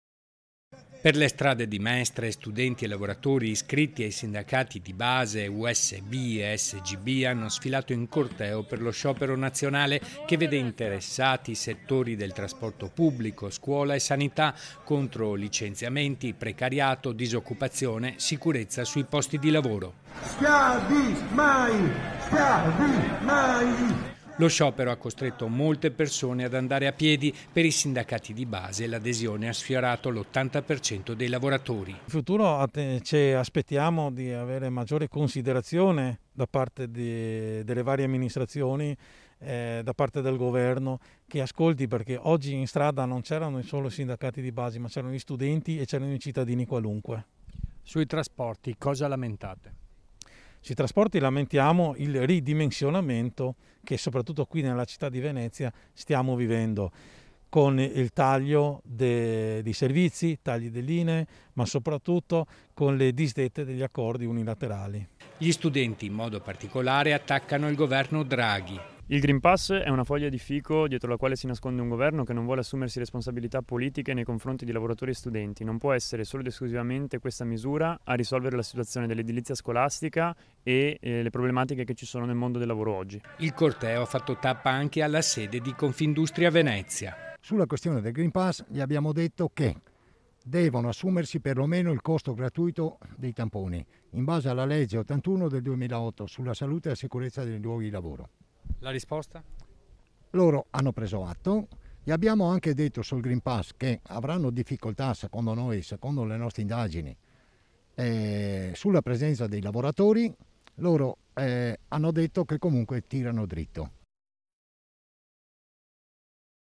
SCIOPERO DEI SINDACATI DI BASE CONTRO LE SCELTE DEL GOVERNO | 11/10/2021
A3 NEWS Venezia 11/10/2021 - MESTRE - Sciopero e corte per le strade di Mestre, nell'ambito della mobilitazione nazionale che include anche trasporti, scuola e sanità. || Per le strade di Mestre studenti e lavoratori iscritti ai sindacati di base Usb e Sgb hanno sfilato in corteo per lo sciopero nazionale, che vede interessati i settori di trasporto pubblico, scuola e sanità, contro: licenziamenti, precariato, disoccupazione, sicurezza sui posti di lavoro.bite dal filmato. Schiavi mai Lo sciopero ha costretto molte persone ad andare a piedi. Per i sindacati di base l'adesione ha sfiorato l'80% dei lavoratori. Lo sciopero ha costretto molte persone ad andare a piedi. Per i sindacati di base l'adesione ha sfiorato l'80% dei lavoratori. Il corteo ha fatto tappa anche alla sede di Confindustria Venezia - Intervistati ALBERTO CANCIAN (USB Lavoro Privato Veneto), TOBIA NAVARRO (Resp. Prov. Fronte Gioventù Comunista
), GINO BORTOLOZZO (USB Lavoro Privato Veneto) (Servizio di Nicola Marcato) - Segui Antenna Tre anche sul digitale terrestre! Visita il sito